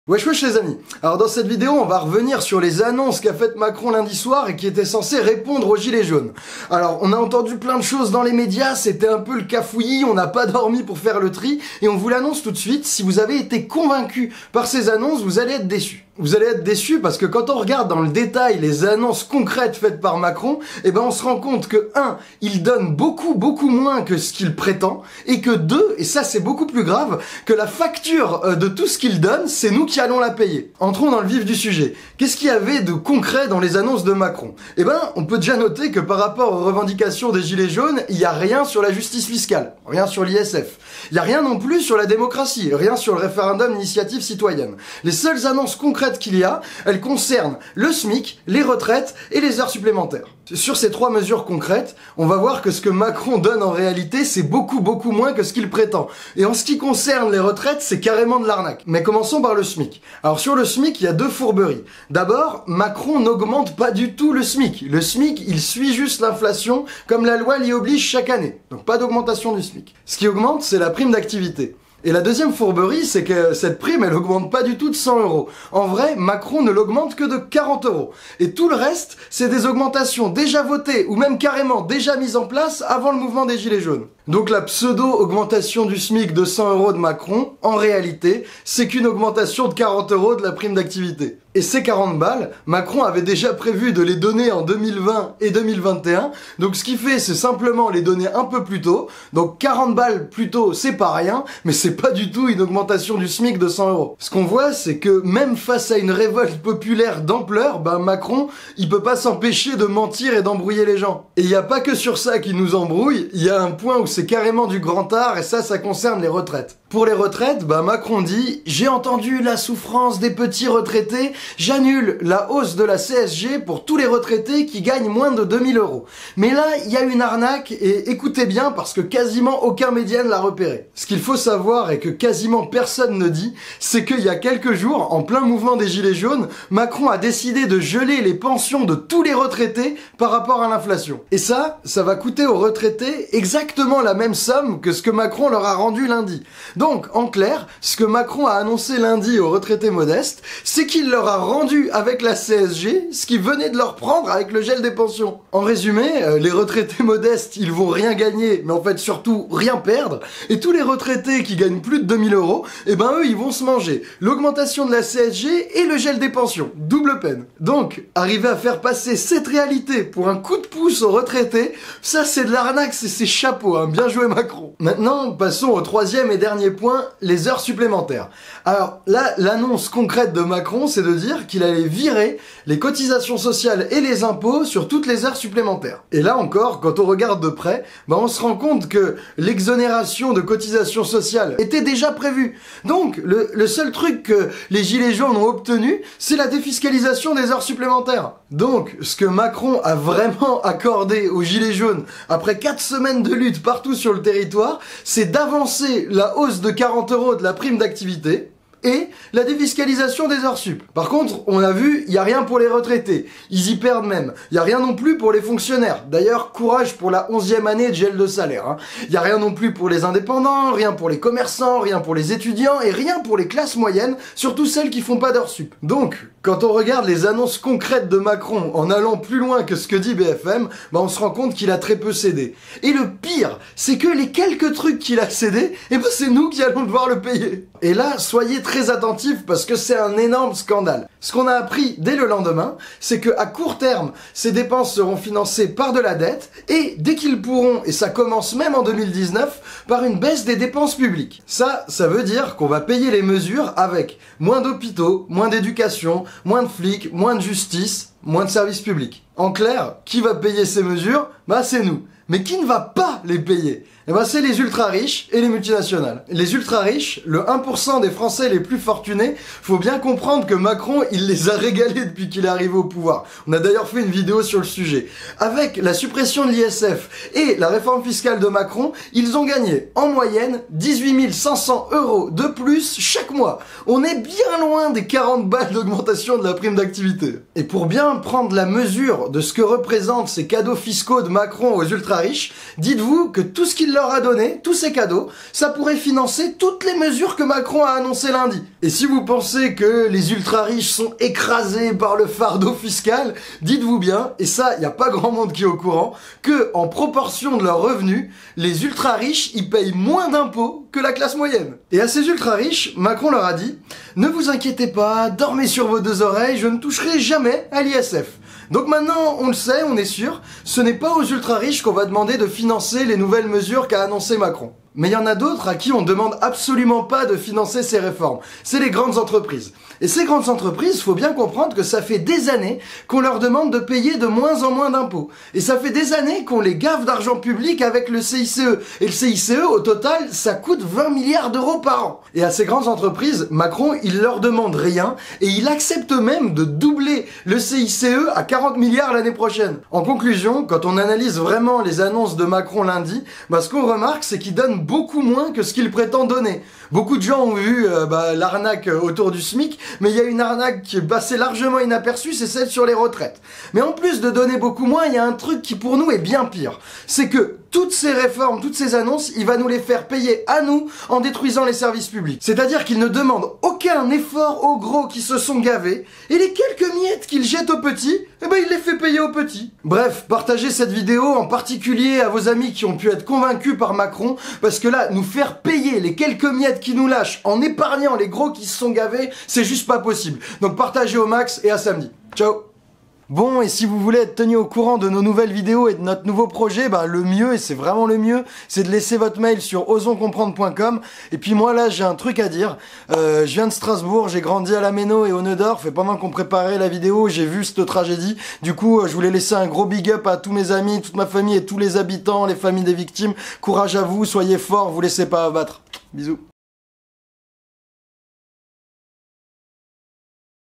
LES MESURES GILETS JAUNES DE MACRON
[ CE QUE MACRON N'A PAS DIT ] Si Macron vous a convaincu, regardez ça. SMIC, retraités, heures sup' ? Les Gilets Jaunes ont-ils été entendus ?

Que valent ses promesses ? Que donne-t-il vraiment ?
SMIC +100 euros ? ✅ CSG rendue aux retraités modestes ? ✅
⛔️ Pas si vite ! ⛔️

Mais surtout, la question à se poser : QUI VA PAYER ? 😒

▶️ découvrez pourquoi le SMIC ne va pas augmenter, et pourquoi vous ne toucherez pas 100 euros💸💸💸

▶️ Découvrez une arnaque d'anthologie sur la CSG des retraités 👵🧓

▶️ Découvrez qui va payer la note. SPOILER : nous.

Bref, avec cette vidéo vous comprendrez pourquoi Macron a tenté d'accélérer encore en gardant le même cap en nous enfumant 😤

Il faut faire payer les multinationales et les plus fortunés dont la richesse a explosé grâce à Macron ✊

ERRATUM : Nous disons que la réforme fiscale fait gagner 18 500 € par mois aux 1% les plus riches. Nous avons fait une ENORME ERREUR. Ils gagnent ces 18 500 € sur une année ! Cela équivaut donc à 1 550 € par mois ! Nous présentons nos excuses pour cette erreur. On était visiblement un peu trop fatigués pour regarder nos propres vidéos.